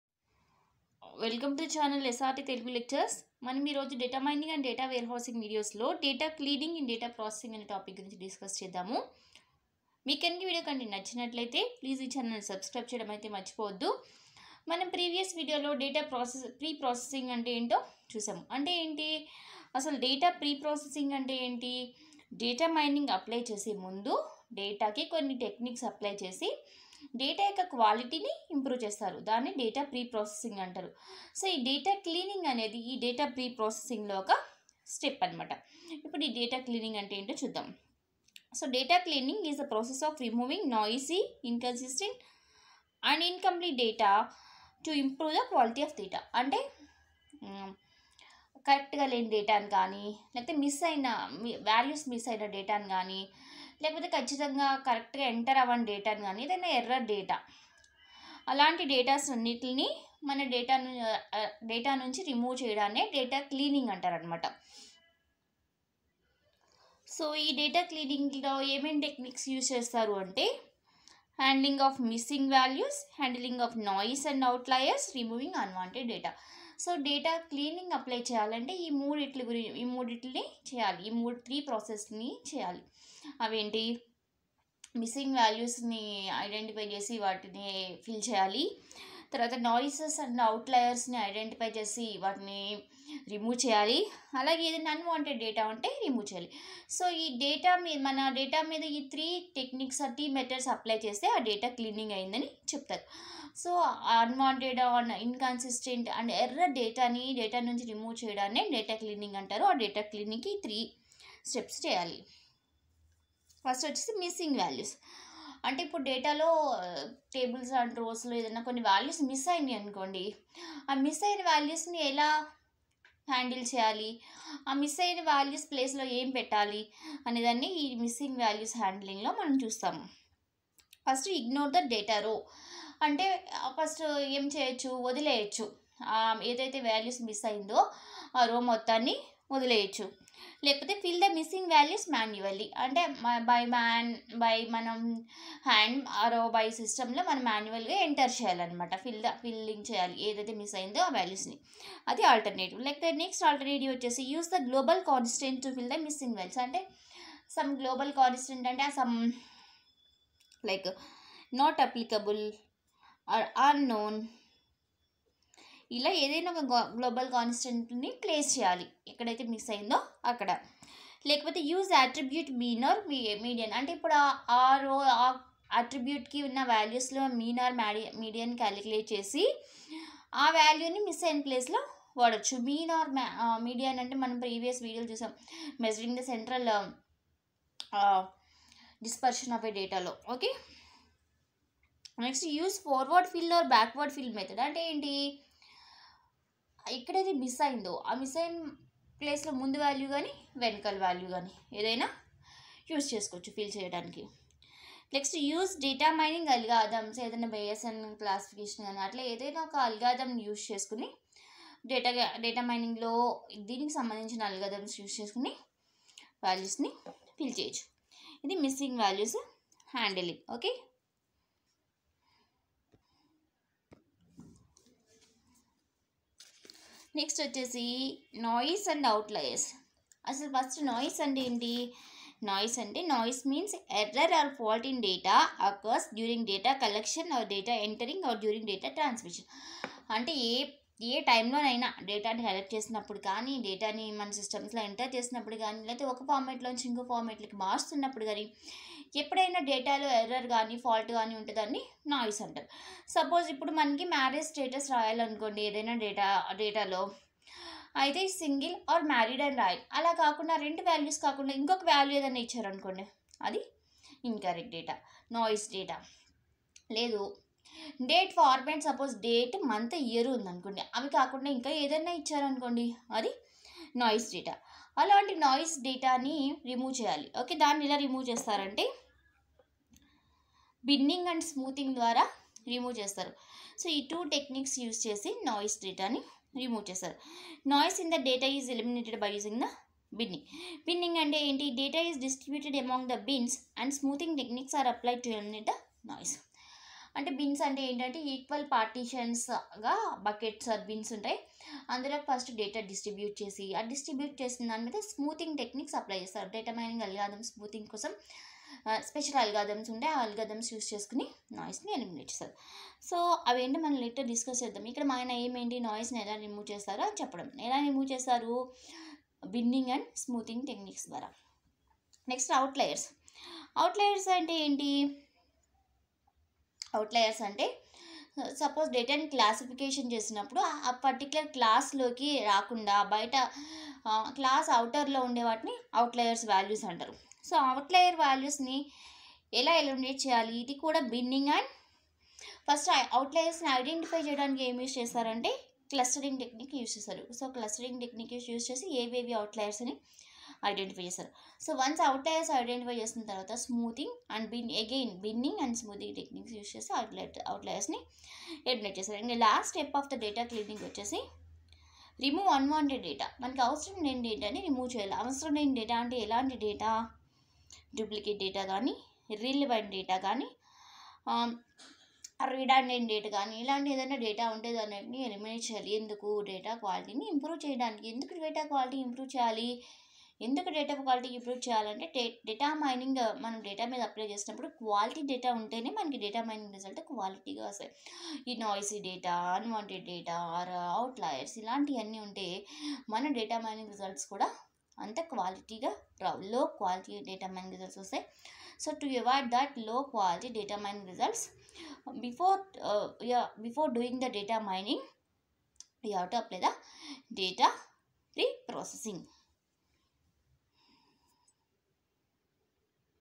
data cleaning in data preprocessing | data mining and data warehouse videos | SRT Telugu Lectures
data cleaning in data preprocessing

steps in data preprocessing

missing values

noise and outliers